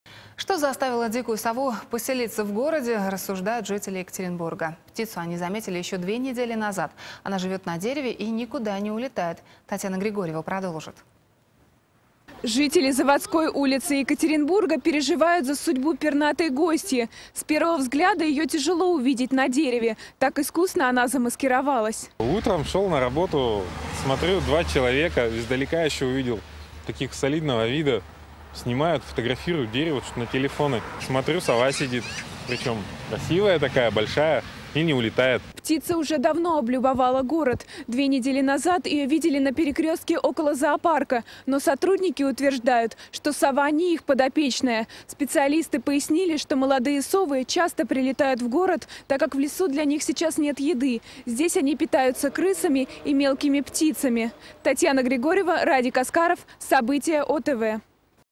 В Екатеринбурге поселилась дикая сова
Что заставило дикую сову поселиться в городе, обсуждают жители Екатеринбурга. Птицу они заметили еще 2 недели назад, она живет на дереве и никуда не улетает.